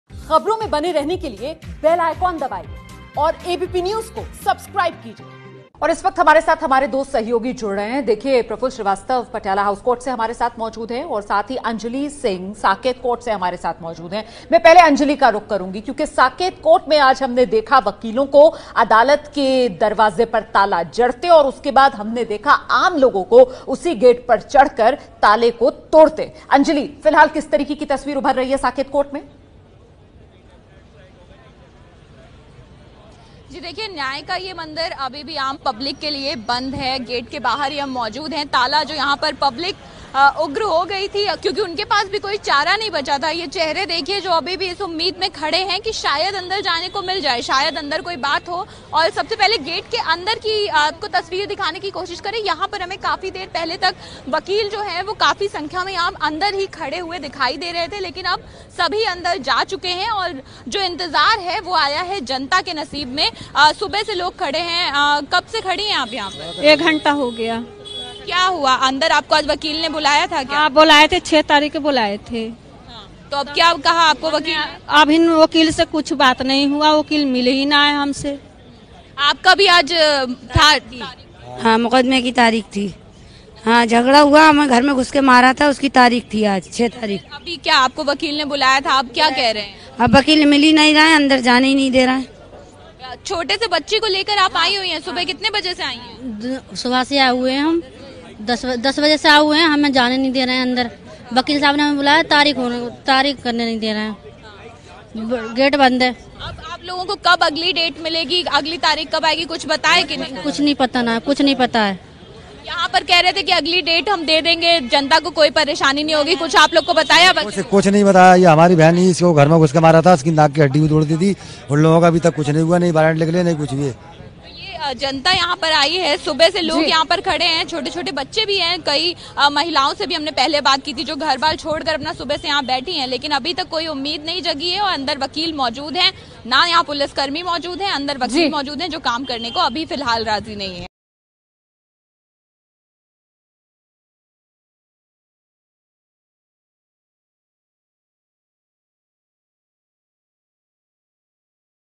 Police vs Lawyer: वकीलों की हड़ताल के बाद साकेत कोर्ट से Ground Report | ABP News Hindi
तीस हजारी कांड के बाद हड़ताल पर बैठे वकीलों ने अपनी मांग रखी है. वकीलों की मांग है कि जिन पुलिसवालों ने गोली चलाई उन पर कार्रवाई हो. 3 बजे जब हाईकोर्ट में सुनवाई हो जाएगी उसके बाद फैसला लेंगे की क्या करना है आगे. जिन लोगों की आज डेट लगी थी कोर्ट में उनको कोई नुकसान नहीं होगा हम नई डेट का चार्ट लगा देंगे जल्द ही.